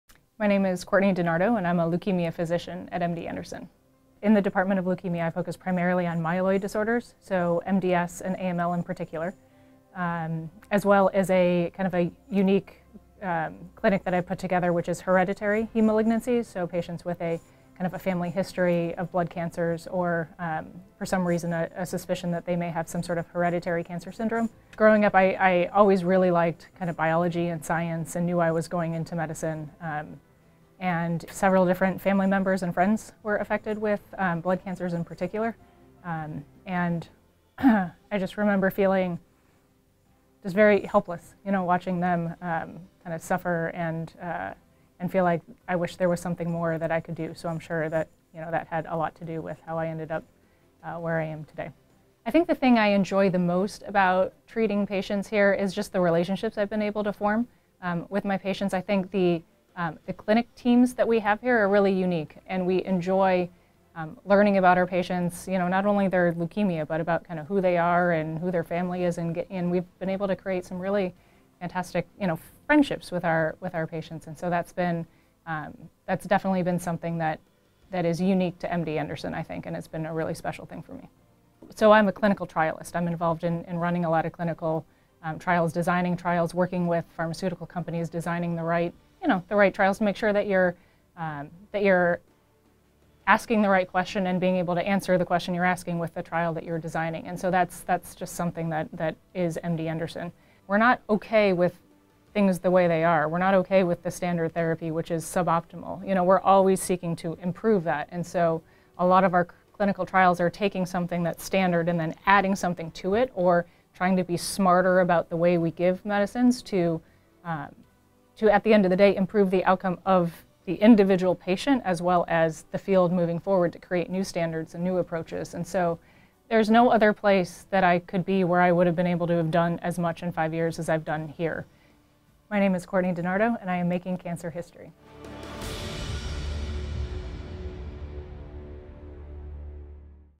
Meet leukemia oncologist Courtney DiNardo, M.D.
Courtney DiNardo, M.D., is an assistant professor in MD Anderson's Leukemia department.

Dr. DiNardo was born and raised in Michigan and earned her medical degree from The University of Michigan, and a master's degree in Clinical Epidemiology and Clinical Trials from The University of Pennsylvania. Her specialty is myelodysplastic syndromes (MDS) and acute myeloid leukemia (AML).

Her goal is to make the standard of treatment for leukemia better, and she hopes to accomplish this through clinical trials, specifically targeted therapies.

She enjoys forming relationships with her patients and staff. Outside of work, her husband and two girls keep her busy.

Request an appointment at MD Anderson by calling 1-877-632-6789